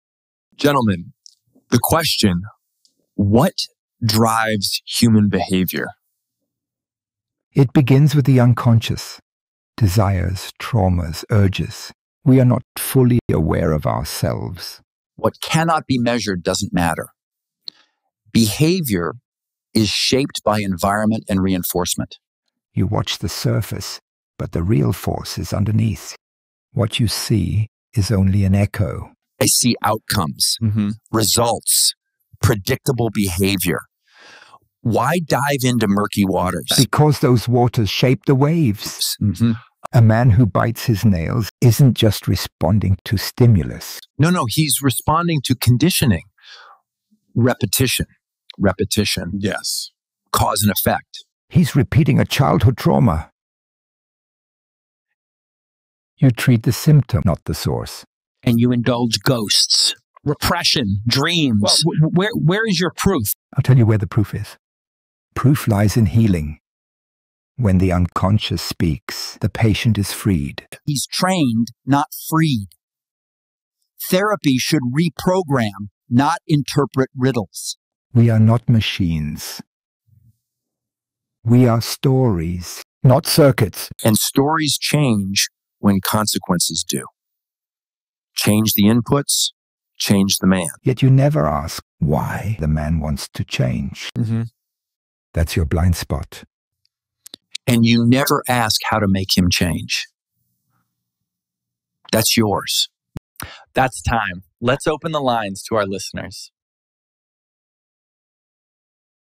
ICONS IN CONFLICT: Sigmund Freud vs B.F. Skinner
The Mind: Unconscious Desires vs. Observable Behavior | Freud vs. Skinner – A Hypothetical Debate

What drives human behavior?
Is it the hidden desires buried deep in our unconscious mind, or the external reinforcements shaping our actions?

Step into a hypothetical intellectual showdown between two titans of psychology—Sigmund Freud, the father of psychoanalysis, and B.F. Skinner, the pioneer of behaviorism. Moderated in a tense and theatrical setting, this debate explores the very essence of human motivation.

🔹 Freud: “We are not machines. We are stories, not circuits.”
🔹 Skinner: “Change the inputs, change the man.”

🔥 In this stylized confrontation:

Freud defends the mysteries of the unconscious: dreams, trauma, repression.

Skinner champions observable behavior: conditioning, reinforcement, data.